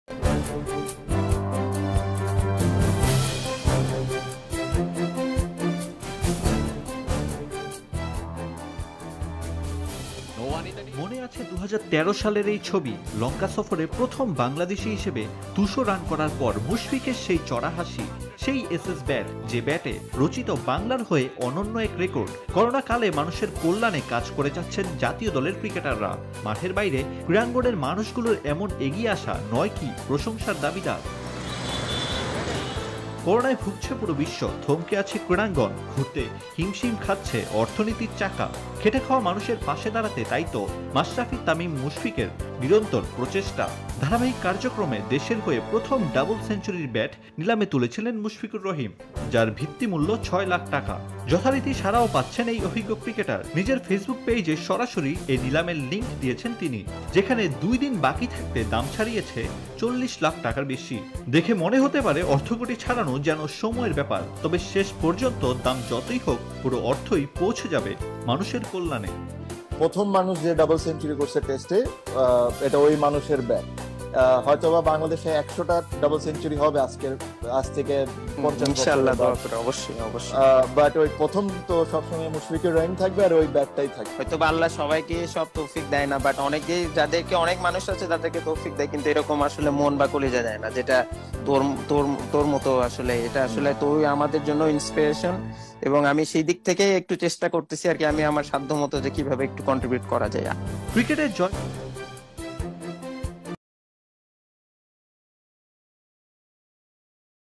মুশফিকের ব্যাটের নিলাম-Channel Bangla News 24 TV- on You tube
টেস্টে স্বাগতিক শ্রীলঙ্কার বিপক্ষে করা নিজের প্রথম ডাবল সেঞ্চুরির ব্যাটটি নিলামে তুলেছেন মুশফিকুর রহিম। করোনায় অসহায় হয়ে পড়া মানুষদের সহযোগিতায় মুশফিকের ব্যাটের নিলাম শুরু হয়ে গেছে গতকাল রাতেই। ছয়দিন ব্যাপী নিলামে ব্যাটটির ভিত্তিমূল্য ধরা হয়েছে ৬ লাখ টাকা।